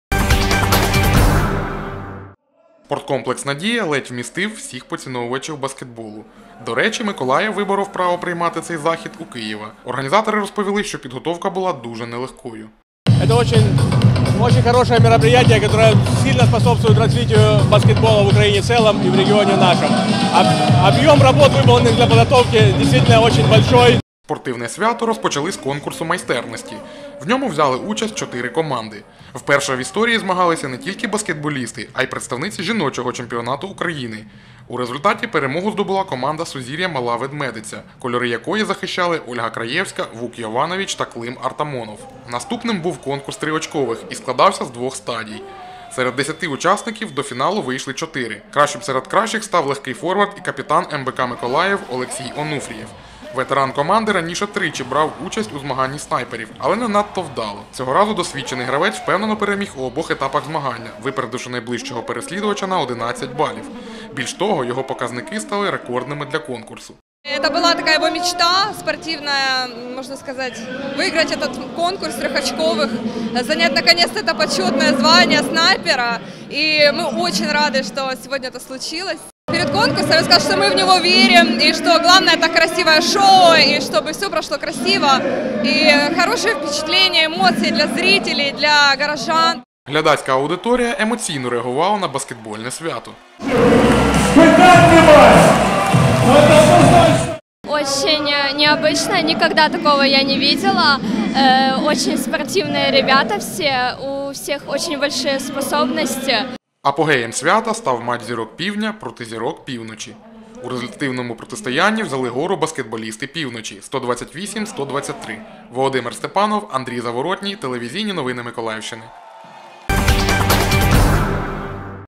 ТНМ — сюжет. Найкращі гравці баскетбольної Суперліги України завітали до Миколаєва
Найкращі гравці баскетбольної Суперліги України завітали до Миколаєва. Домашня арена МБК “Миколаїв” приймала зірок втретє. Яким було це видовище — дивіться в нашому репортажі.